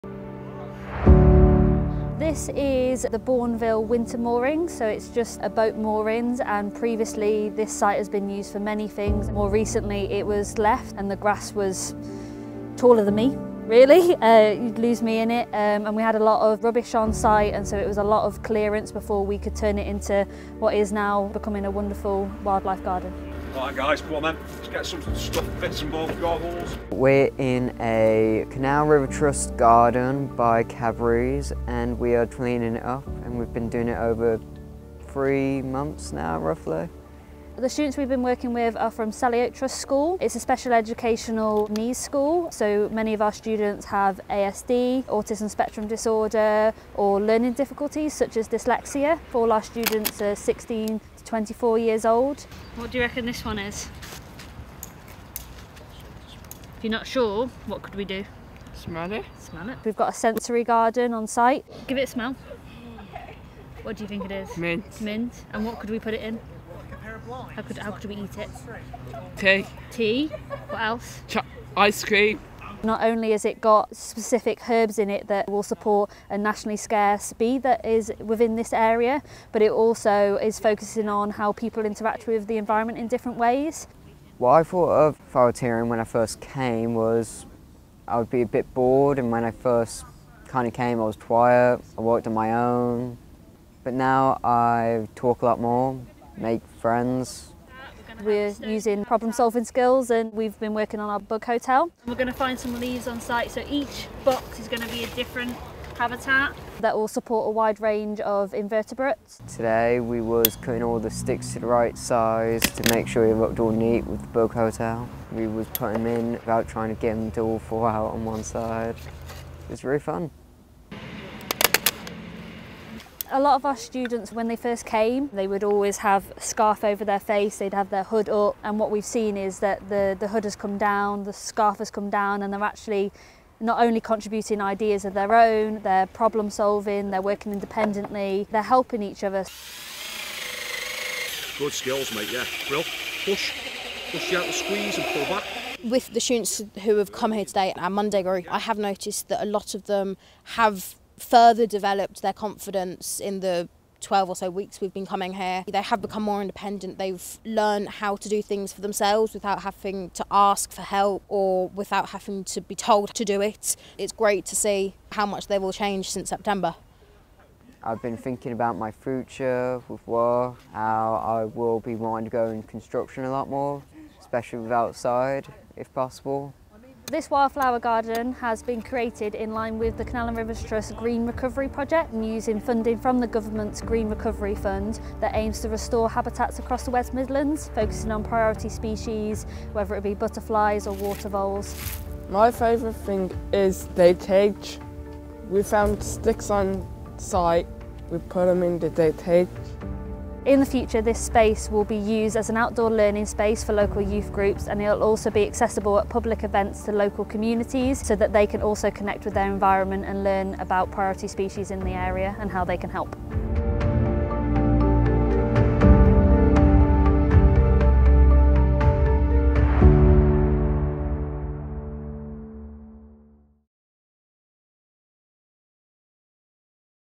Bournville Wildlife Garden
As part of our Green Recovery Fund project, we've created a new nature garden at Bournville Moorings in the West Midlands.